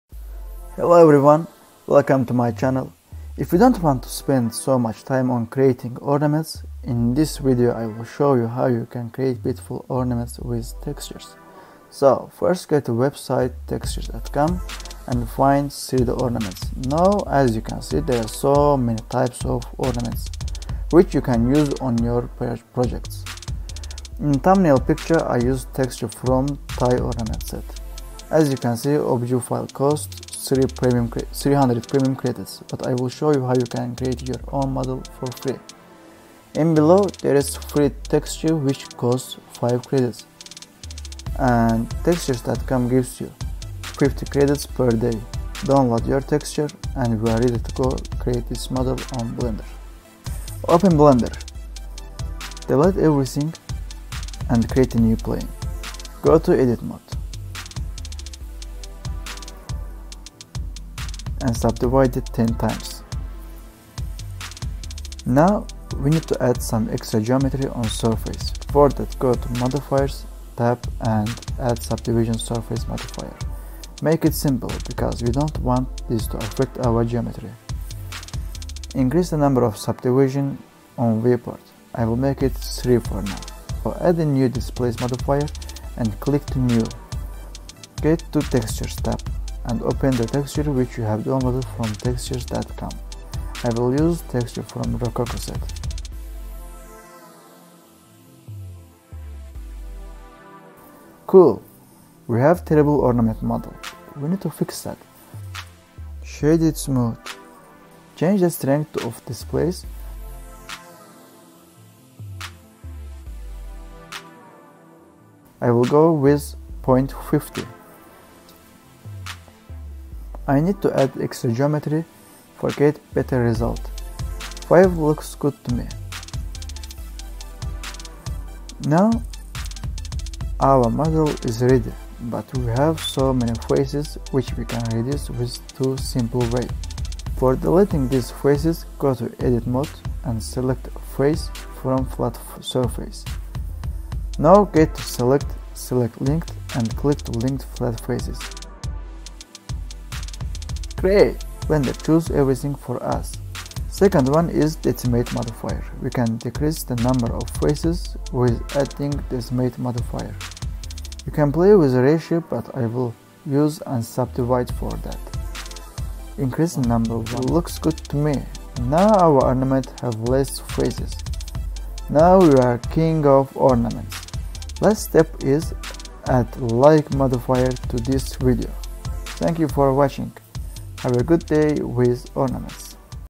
Blender - Quick Way to Create Ornaments in 3 minutes
Blender - Create models of ornaments with a simple way.
Learn How you can create models of ornaments in blenders with textures.